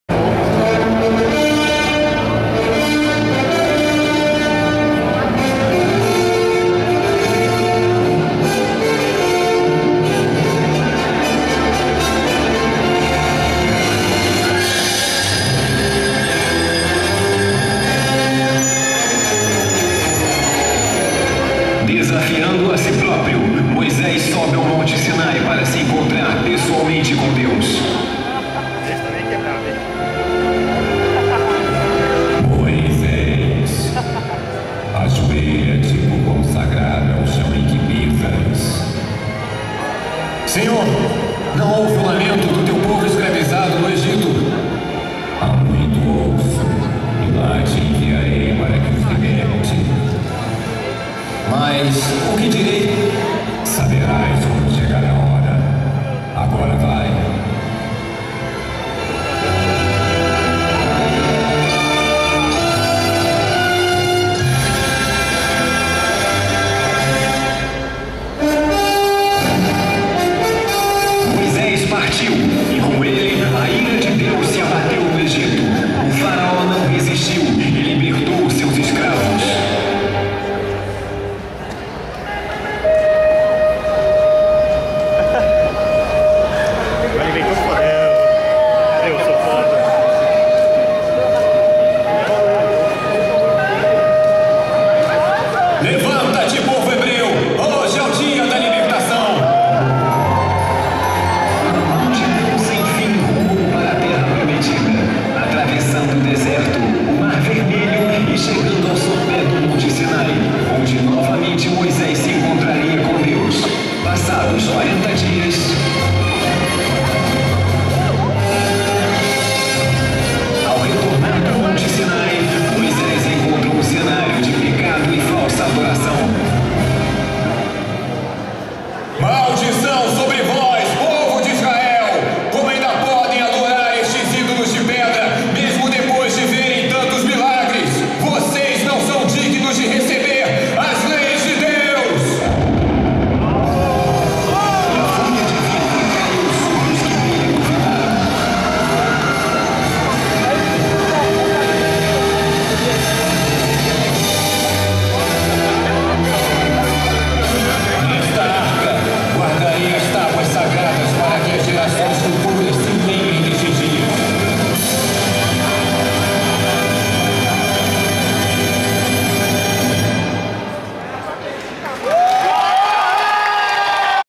Anime Friends 2011 - YCC - Moisés
Apresentação Tradicional do YCC da Yamato dia 15/07/2011 no evento AnimeFriends ocorrido em São Paulo no Mart Center.